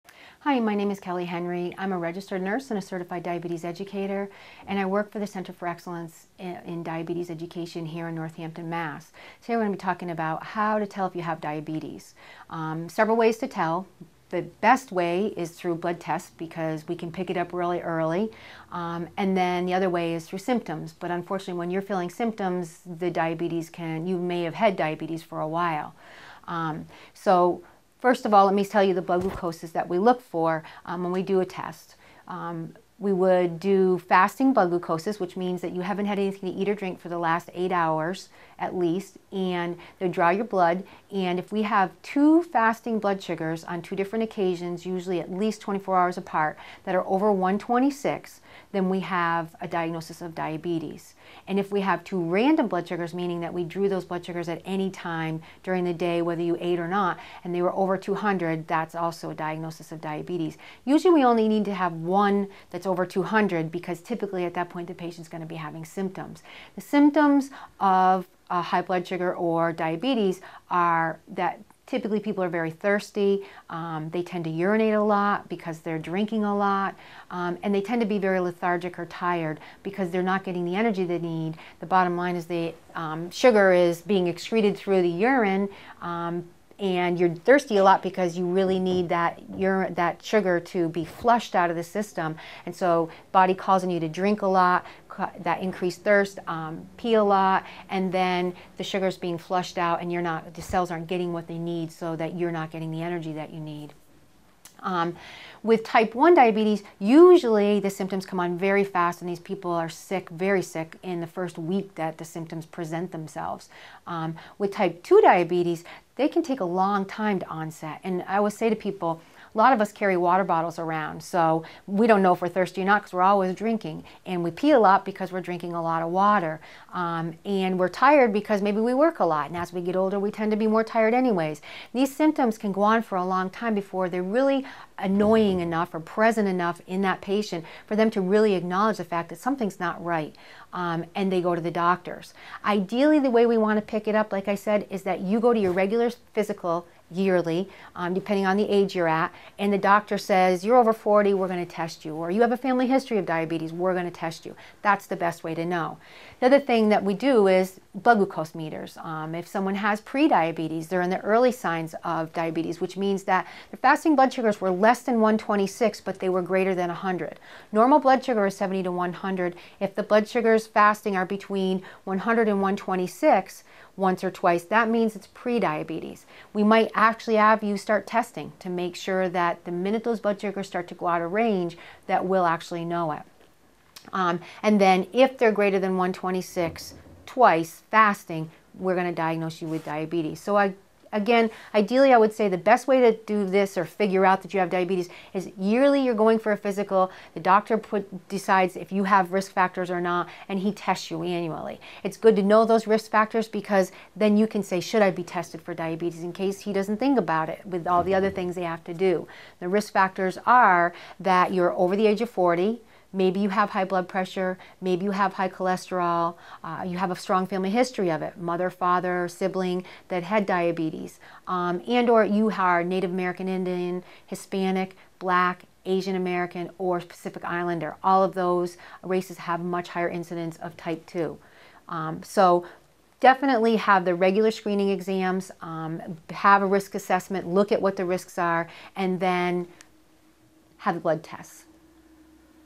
Diabetes Tips : How to Tell If You Have Diabetes
The best way to tell if you have diabetes is through blood tests, which can pick up diabetes the earliest. Have your blood checked with advice from a registered nurse in this free video on diabetes.

Expert: Kelly Henry, RN, CDE
Bio: Kelly Henry has been a registered nurse for more than 25 years and a certified diabetes educator for more than 13 years.